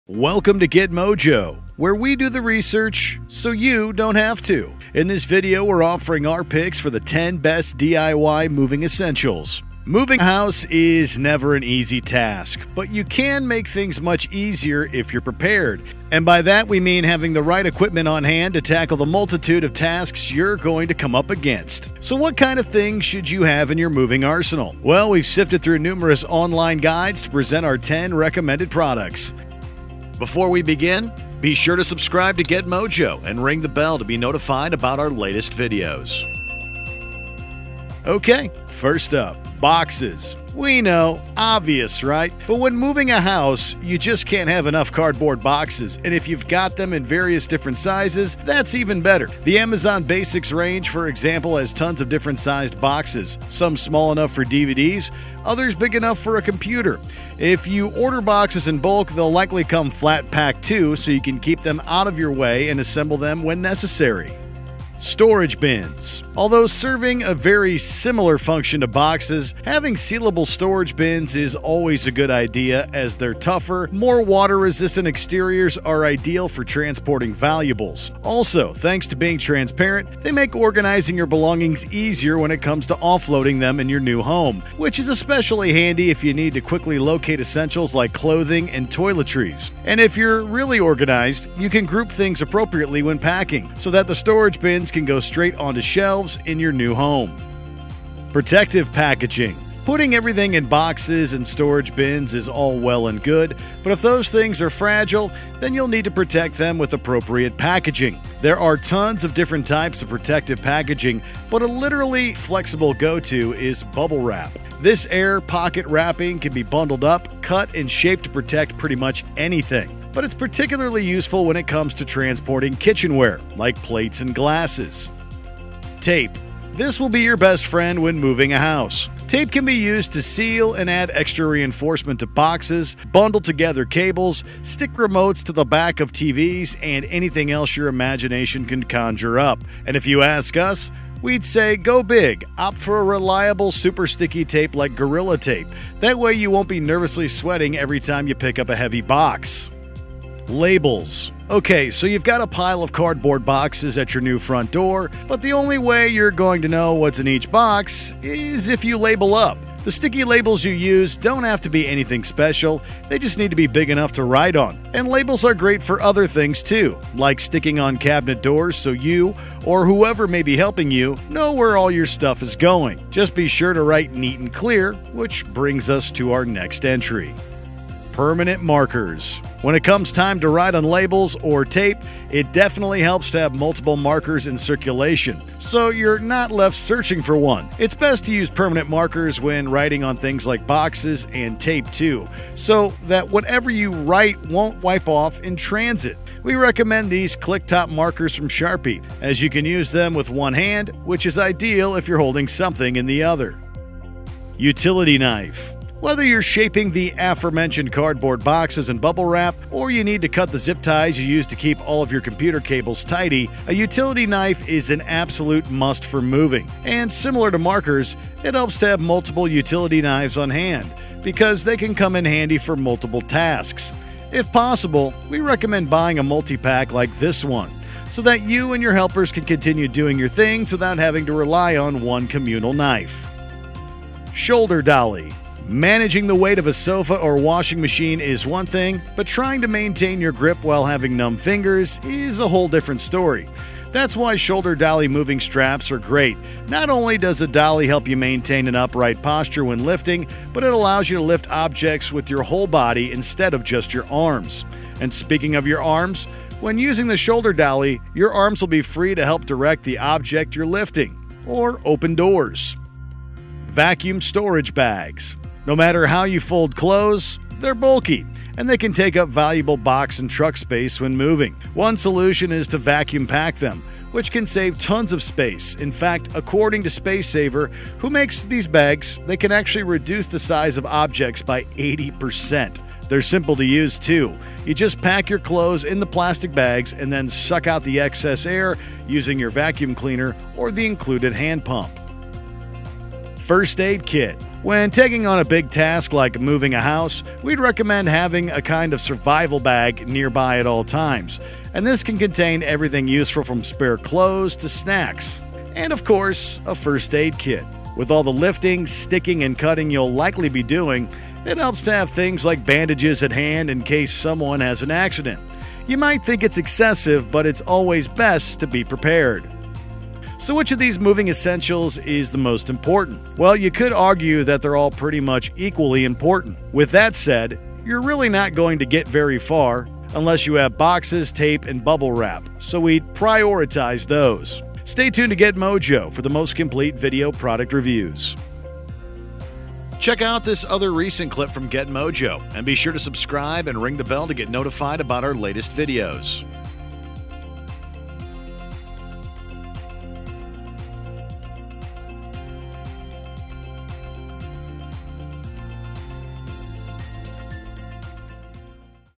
Top 10 DIY Moving Essentials
Moving house is never an easy task. But you can make things much easier if you’re prepared. And by that, we mean having the right equipment to hand to tackle the multitude of tasks you’re going to come up against. So what kind of things should you have in your moving arsenal? Well, we’ve sifted through numerous online guides to present our 10 recommended products.